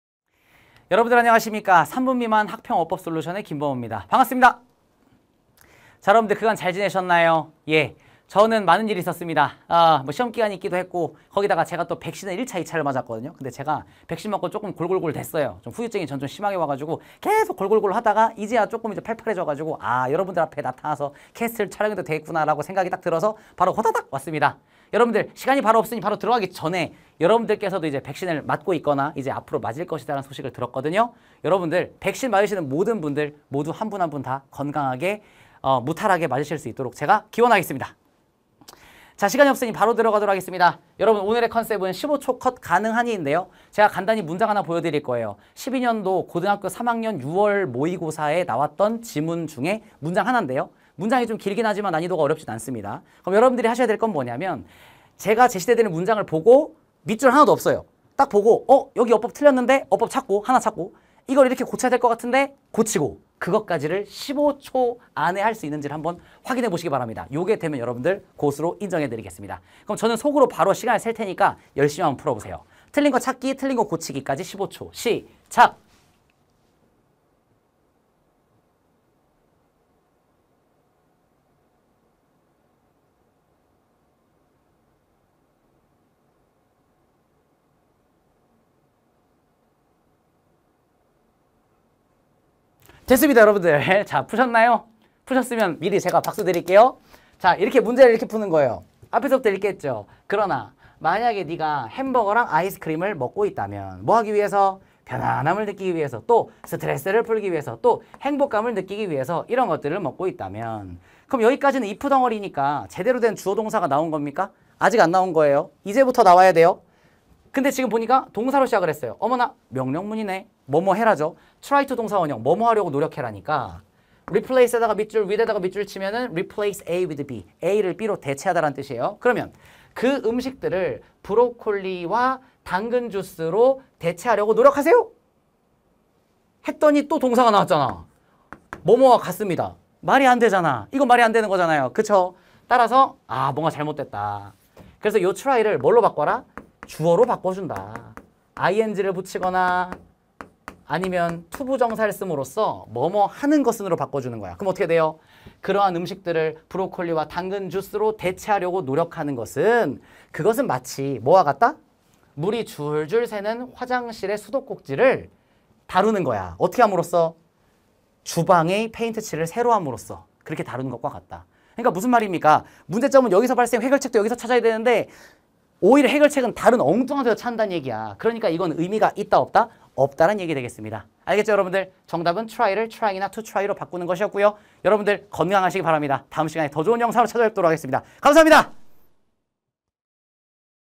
[메가스터디] 영어 김범우쌤 - [3분 미만] 15초 컷 가능?
오랜만에 돌아온
[3분 미만] 어법 솔루션!
(하지만 10초가 넘어가버린..)

여러분은 문장 속 틀린 어법을 찾고,
틀린 것을 올바르게 고치기까지
15초 안에 가능하신가요?

실제 모의고사에 나온 문장으로
범우쌤과 함께 테스트 해보고
여러분의 실력을 점검해보세요~

15초 컷 가능한지 테스트 해보고 싶다면?

지금 바로 영상 재생 GO!

- 영어 김범우 선생님에 대해 더 자세히 알고싶다면?